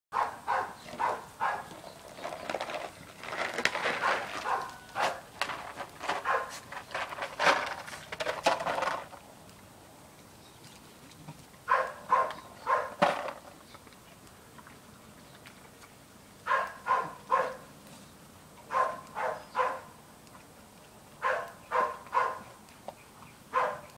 How much fun an empty cordial bottle filled with dry bikis
Staffords having fun